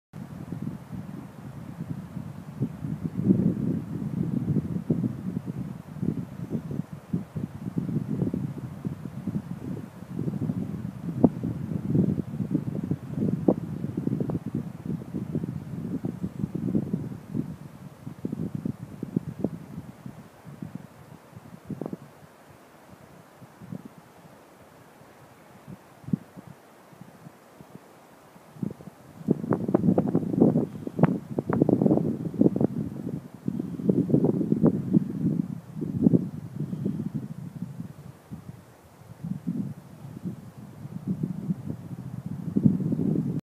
Palanut mökki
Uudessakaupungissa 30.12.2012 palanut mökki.